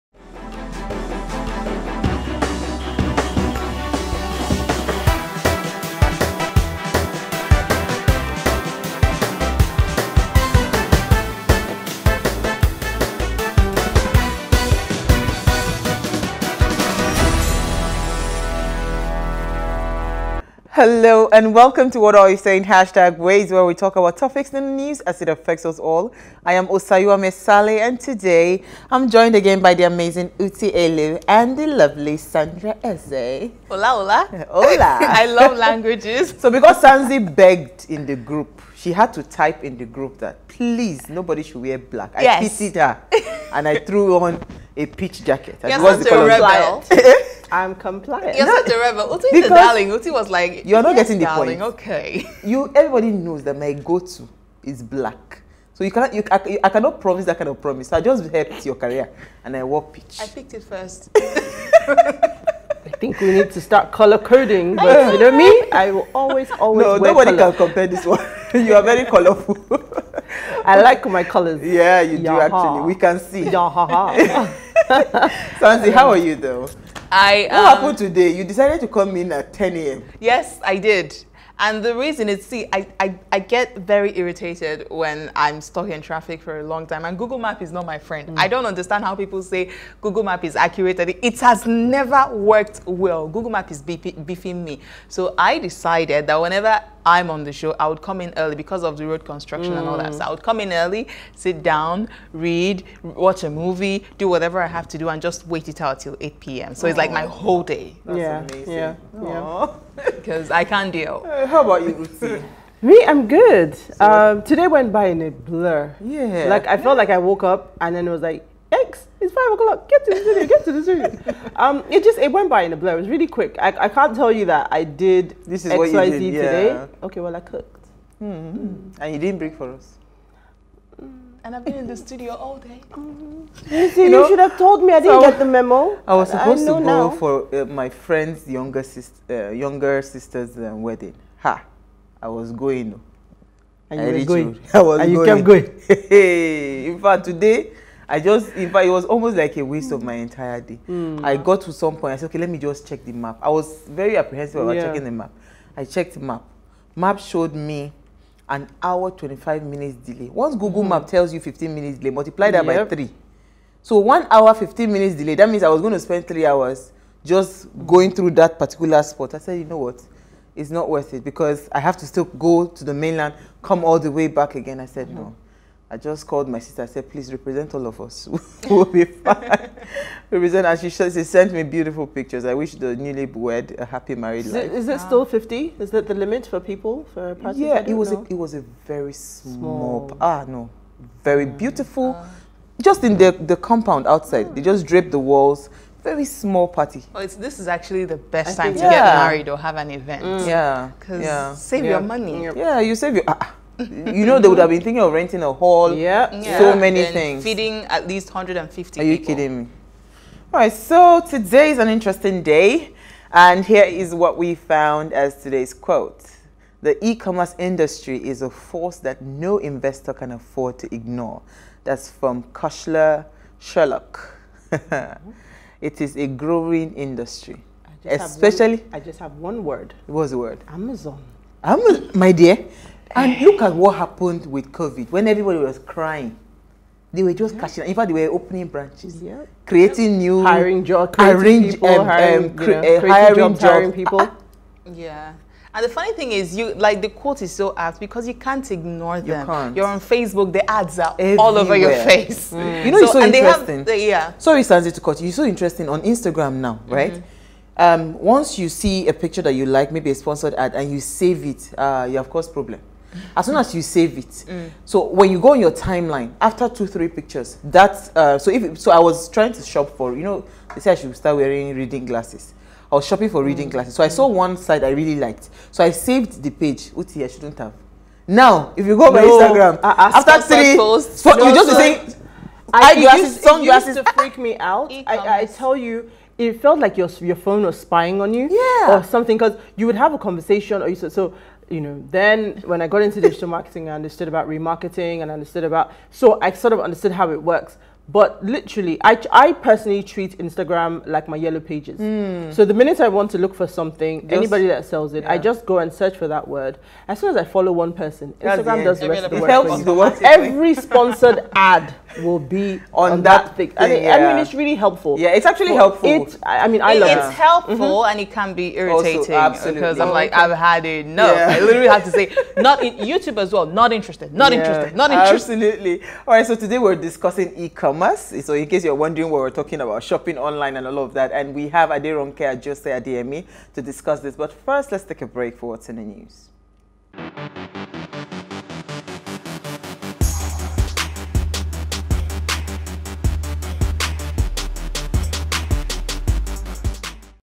The E-Commerce Industry
The WAYS ladies are here again to discuss the issues affecting us. The quote for today is
"the e-commerce industry is a force that no investor can afford to ignore" -  Cushla Sherlock